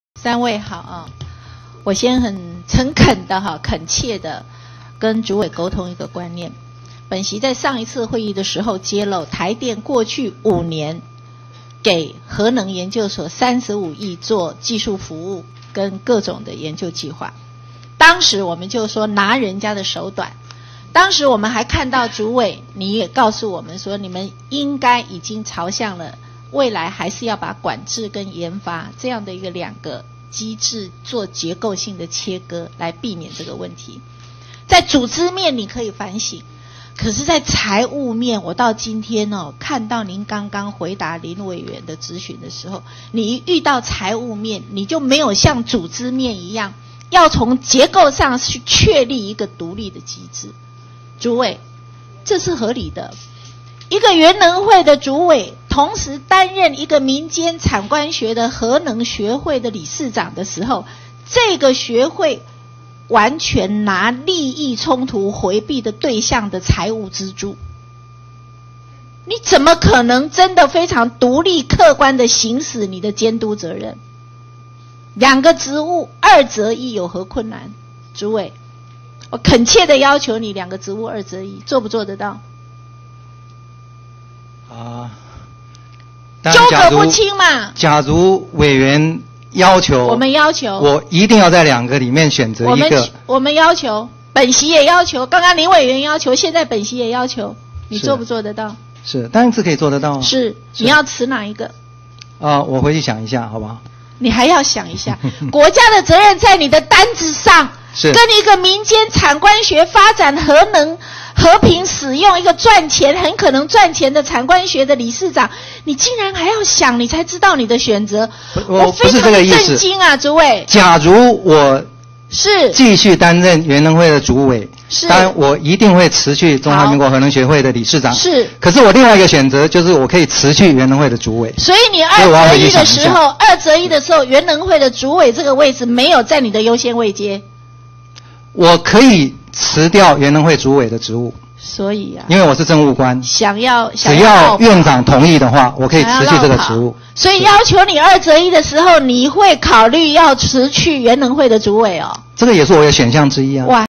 20110418管碧玲要求蔡春鴻在原能會與核能學會二擇一 蔡春鴻：回去想一下
管委員碧玲：主席、各位列席官員、各位同仁。我先很誠懇、懇切的和主委溝通一個觀念，本席在上次會議揭露，台電過去 5 年給核能研究所 35 億元作技術服務和各種研究計畫，當時我們就說：拿人手短。當時主委也告訴我們，你們未來應該會朝向將管制、研發兩個機制做結構性的切割，以避免這個問題。在組織面你可以反省，但到今天，我看到你回答林委員的質詢，一遇到財務面，你就無法像組織面一般，從結構上確立一個獨立機制。這是合理的懷疑，原能會主委同時擔任民間產官學核能學會理事長，這個學會的財務資助完全來自利益衝突迴避的對象，所以你怎麼可能真的非常獨立、客觀行使你的監督責任？兩個職務二擇一有何困難？本席懇切要求主委兩個職務二擇一，你能不能做到？因為這中間糾葛不清。

主席：請原能會蔡主任委員答復。

蔡主任委員春鴻：主席、各位委員。假如委員要求，我一定要在兩個職務選擇一個。

管委員碧玲：我們要求，剛才林委員要求，現在本席也要求，你能不能做到？

蔡主任委員春鴻：當然可以做到。

管委員碧玲：你要辭哪一個職務？

蔡主任委員春鴻：我回去想一下，好不好？

管委員碧玲：你還要想一下？國家的責任在你的擔子上，一個民間產官學發展核能和平使用，而且很可能賺錢的學會，你擔任這個學會的理事長。但你竟然還要想才知道你的選擇，我非常震驚。

蔡主任委員春鴻：我不是這個意思，假如我繼續擔任原能會的主委，我一定會辭去中華民國核能學會的理事長，可是我另外一個選擇是我可以辭去原能會主委。所以我要回去想一想。

管委員碧玲：所以在二擇一的時候，原能會主委的位置沒有在你的優先位階？

蔡主任委員春鴻：我可以辭掉原能會主委的職務，因為我是政務官，只要院長同意，我可以辭去這個職務。

管委員碧玲：所以你想要落跑？要求你二擇一的時候，你會考慮辭去原能會主委？

蔡主任委員春鴻：這也是我的選項之一。